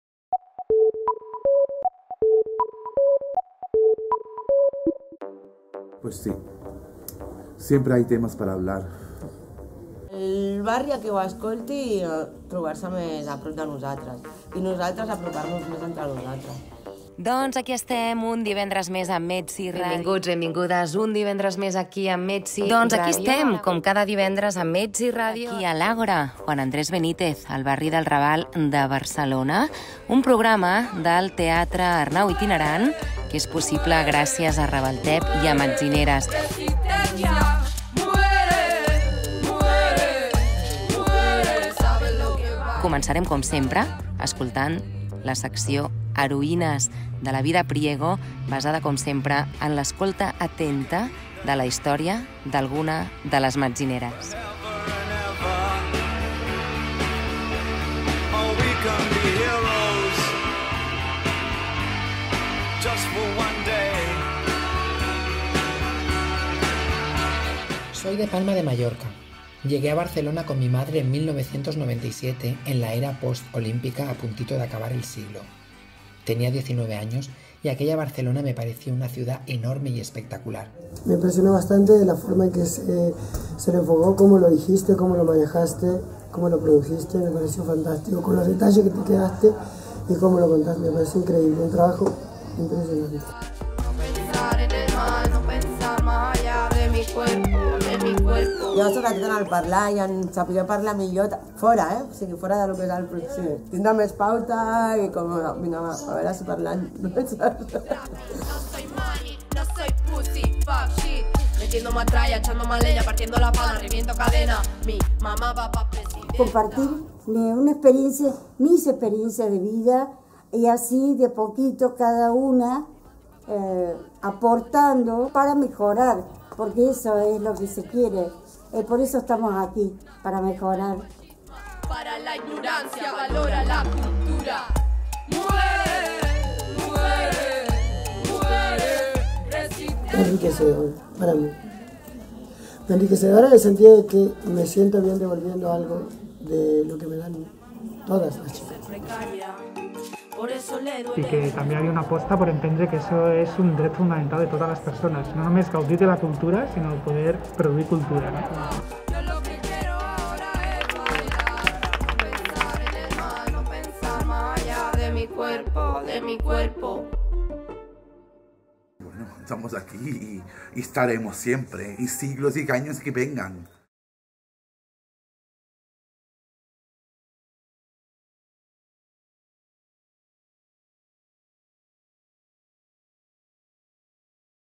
Teaser MetziRadio
Els últims mesos hem estat submergides en les profunditats radiofòniques. Dins del programa del Teatre Arnau Itinerant 20-21 hem tingut l'oportunitat de crear la MetziRadio, un laboratori sociocultural que ens ha servit d'altaveu a moltes mujerxs supervivents.
No et perdis aquest teaser realitzat per Carmen Molina [@carmencapta], viu l'essència de la MetziRadio i no t'oblidis d'escoltar tots els programes, disponibles en YouTube i iVoox!

Temàtiques de la MetziRadio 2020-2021 amb el TeatreArnauItinerant
· Violència contra les donxs
· Dret a la salut
· Sensellarisme
· Presó
· Treball sexual
· Identitats transgènere
· El Raval
· Maternitats
· Interseccionalitat
· Drogues

Els programes són en català i  castellà.

MetziRadio, un programa de l'Arnau Itinerant amb les Metzineres, Ravaltep, Vidda Priego i Marta Molina. T'esperem en la ràdio!